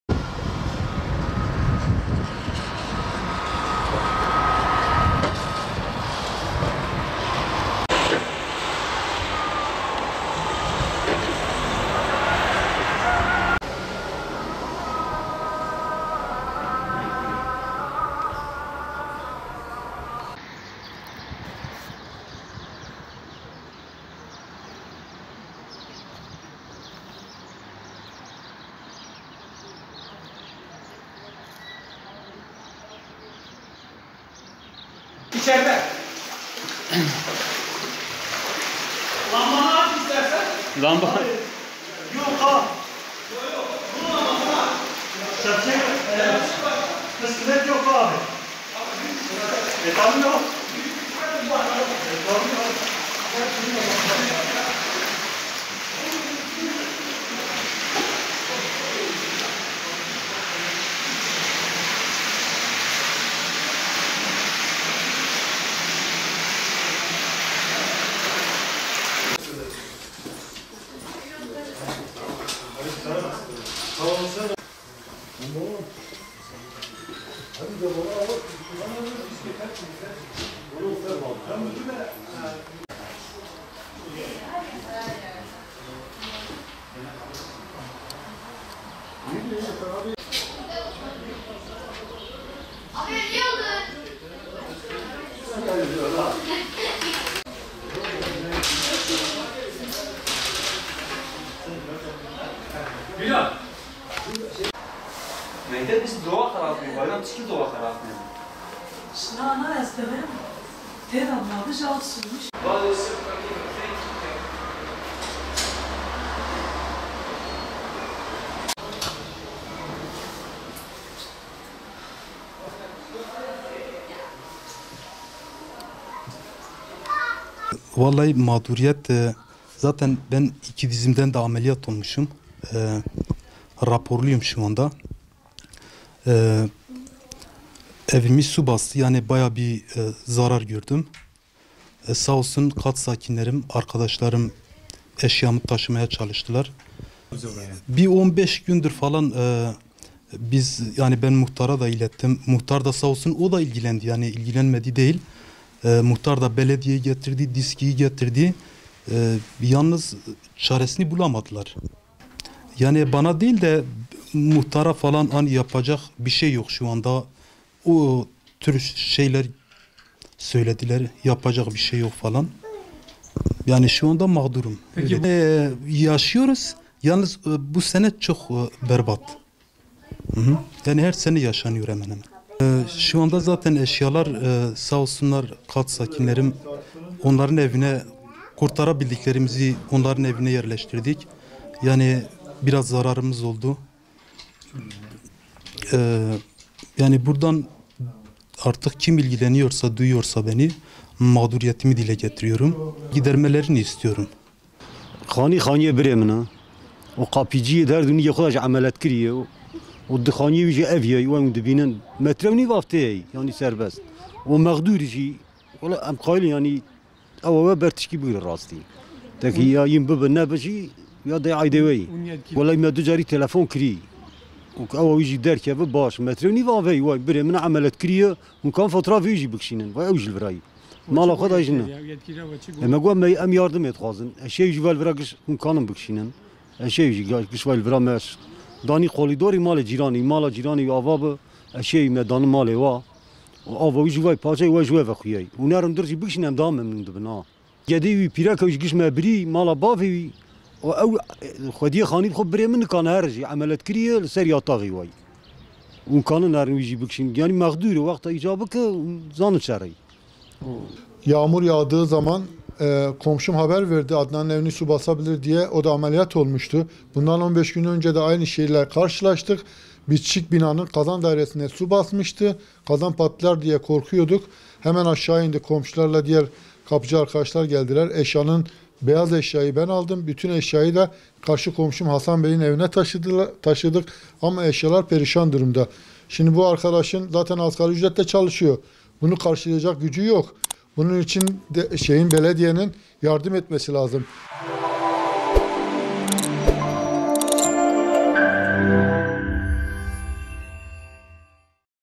Diyarbakır'da evleri sular altında kalan aile yardım bekliyor
Günlerdir etkili olan sağanak yağış, altyapı bakımından yetersiz olan yerlerde hayatı olumsuz etkilerken birçok yerin de sular altında kalmasına neden oldu.

Değerli İzleyicilerimiz;

TÜRKÇE

KÜRTÇE-KURMANCI

ARAPÇA

İNGİLİZCE

FARSÇA

KÜRTÇE-SORANİ